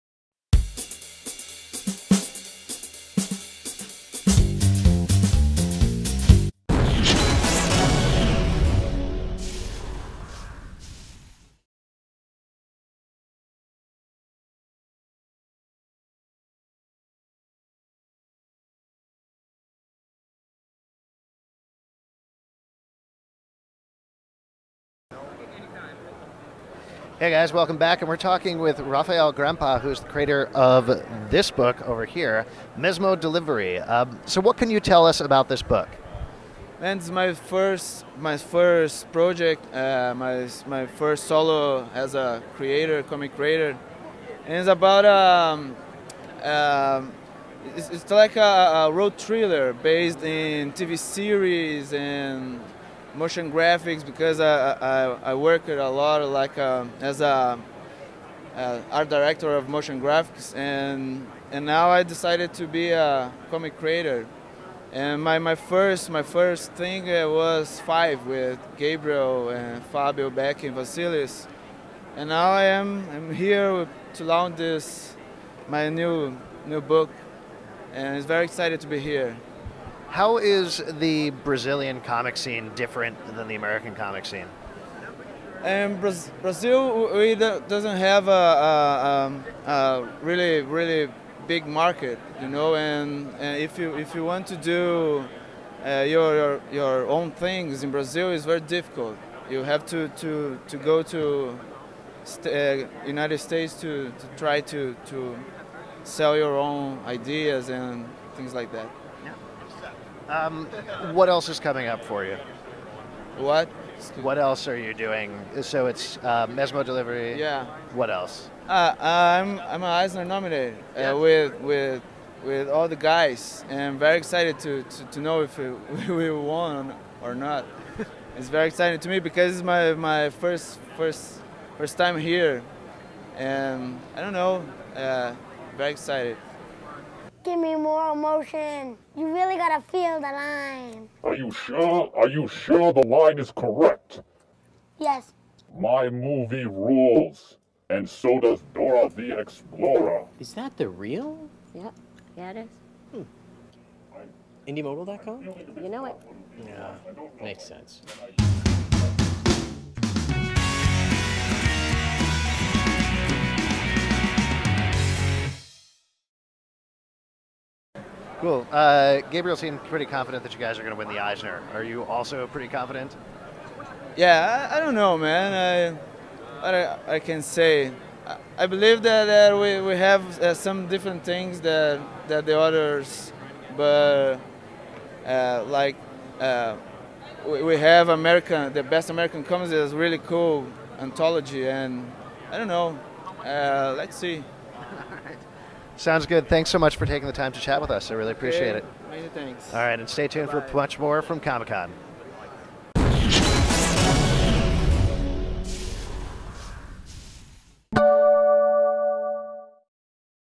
Mesmo Delivery with Eisner Nominee Rafael Grampa
In this special episode of The Stack Alex talks with 2008 Eisner Nominee Rafael Grampa.  First Rafael talks about his origins in motion graphics comics and the journey to creating his own comic book.  Next he and Alex talk about his recent Eisner Nomination for Best Anthology.  Finishing off Rafael talks about the unique qualities of international comics and why he thinks they're better than ever now.  Be sure to check out Mesmo Delivery!